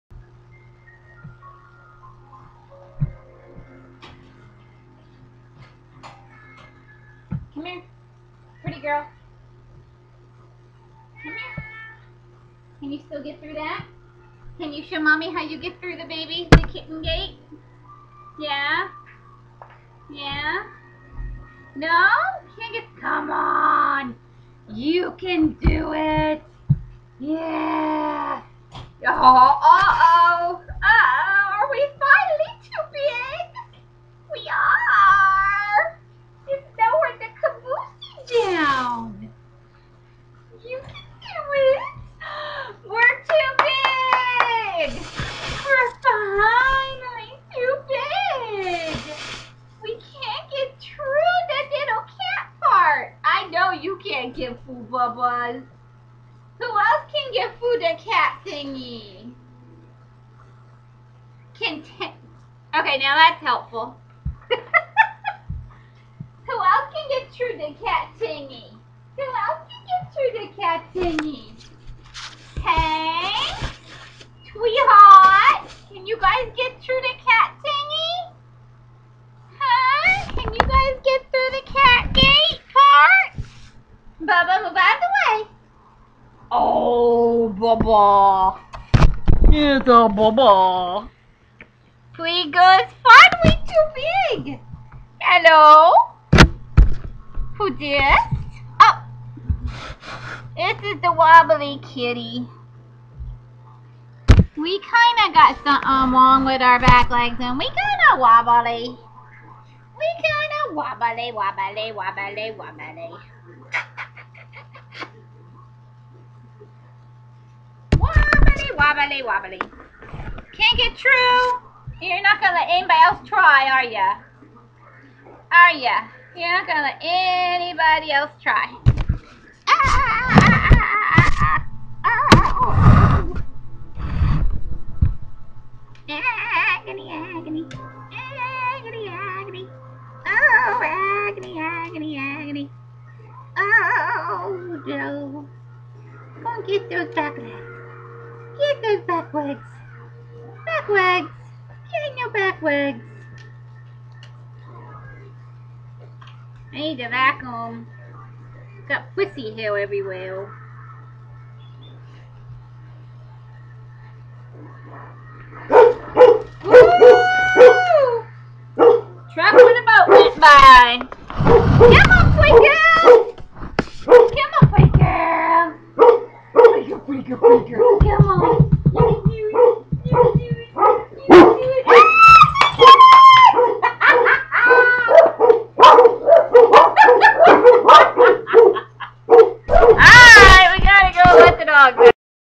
Pretty Girl vs the Cat Gate Door
Have a tall metal gate to keep the dogs in the living room. There's a little door in it for cats to pass through. But someone still fit. Think she still does! Needless to say, that door is kept closed and the cats get out another way. And, yes, I do talk to my animals like this but my voice only sounds like Minnie Mouse when recorded. Honest. Even I found the sound of my voice annoying!